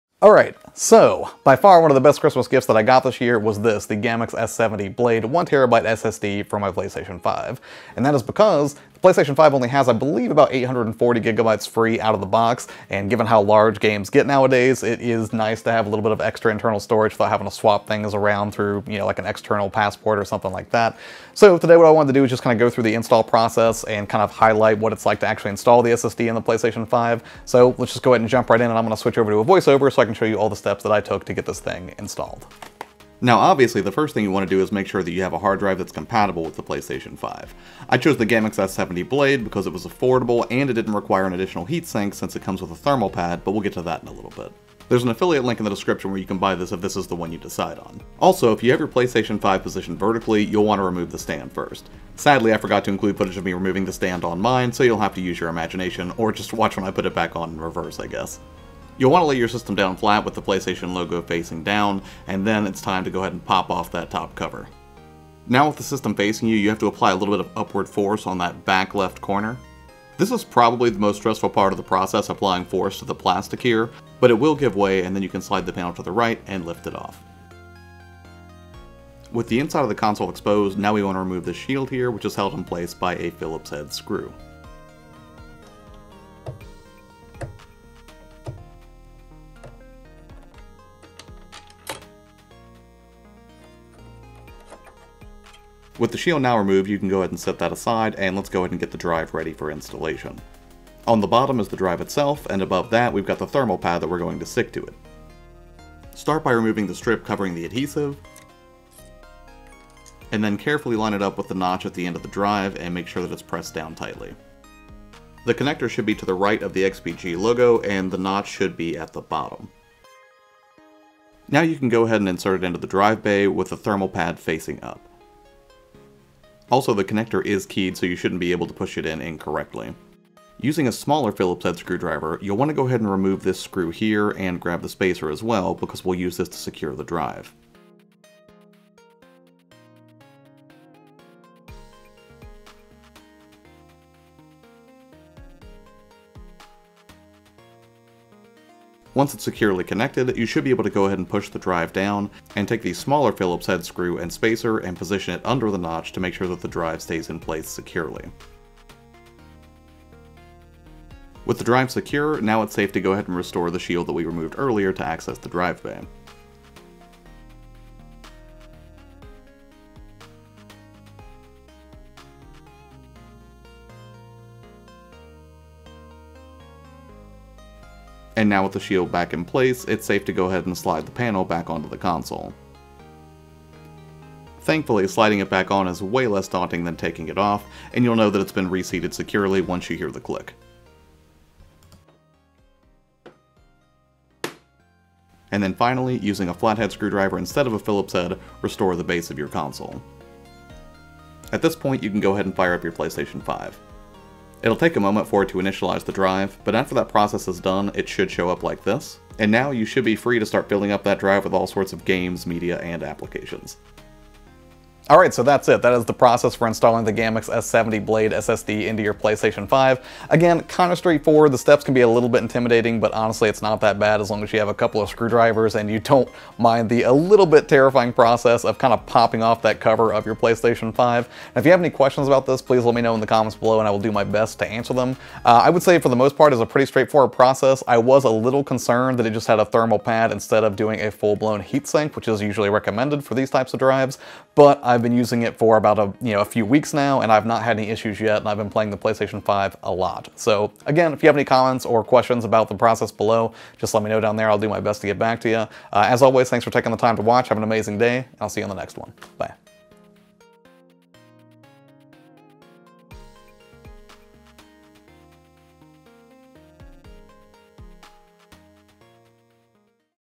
Gammix S70 SSD PS5 Installation Guide
If you're thinking of adding more storage to your PS5, here's my Gammix S70 SSD PS5 installation guide. If you have any questions or suggestions on how I can improve future tutorials, please let me know since this is kind of new territory for me!

0:00 - Intro
0:32 - Prepping the System
1:01 - Removing the Panel
1:17 - Accessing the Drive Bay
1:35 - Applying the Thermal Pad
2:00 - Connecting and securing the SSD
2:40 - Putting the PS5 Back Together
3:20 - Powering Up and Initializing the Drive
3:34 - Thoughts on the Process/Outro